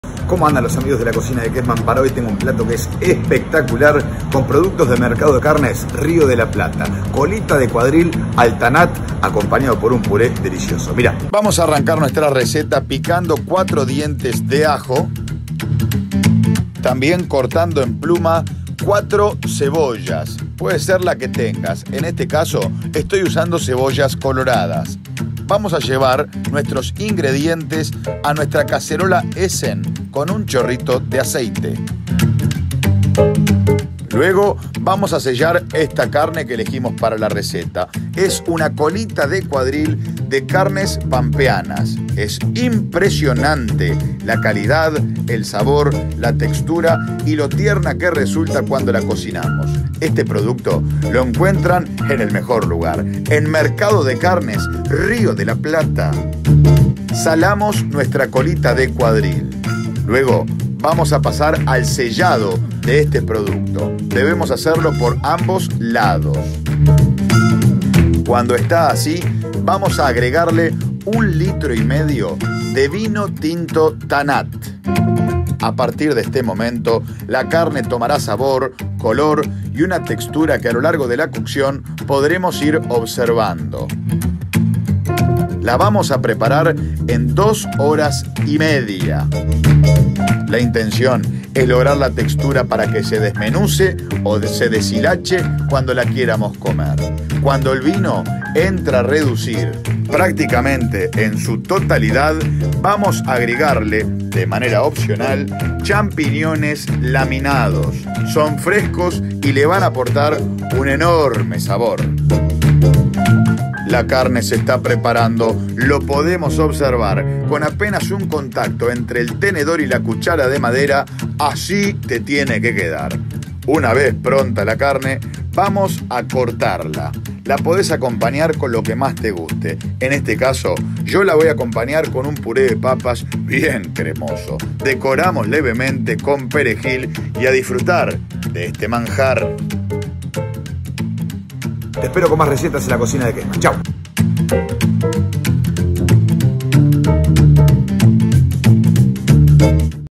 CARNE AL VINO TINTO
Amigos como están!!

Tengo una receta de esas que me gustan a mi.

Una Colita de cuadril al tannat, la haremos en dos horas y media. 🔥🔥

Cocción larga como más me gusta. Para asegurarnos muchísimo sabor en la preparación.

Además estrené mi cacerola @essen.uy que me brindó @essen_silvana la vendedora número uno de estos productos!! 🥘🍲🍲🥘

Aquí les dejo los pasos para la receta!!

📝 Ingredientes 📝

📌 1,5 kg de Colita de Cuadril @carnes_riodelaplata
📌 1,5 L de Vino tinto TANNAT
📌 4 Cebollas coloradas
📌 4 dientes de Ajo
📌 Sal
📌 Pimienta
📌 Hongos
📌 Romero (opcional)

Procedimiento 📝

👉 Ubicar en cacerola un chorrito de aceite. Colocar los ajos picados y las cebollas en pluma

👉 Agregar la Colita de cuadril ya salado para que se selle de los dos lados.

👉 Una vez que estás selladas, agregamos el vino tinto tannat

👉 Cuando el vino redujo, le agregamos los hongos y unas ramitas de romero

👉 Cuando con cuchara de madera y tenedor tocamos la carne y está se desmenuza, está pronta para servir.

👉 Podes acompañar con lo que más te guste. En este caso optamos por puré de papas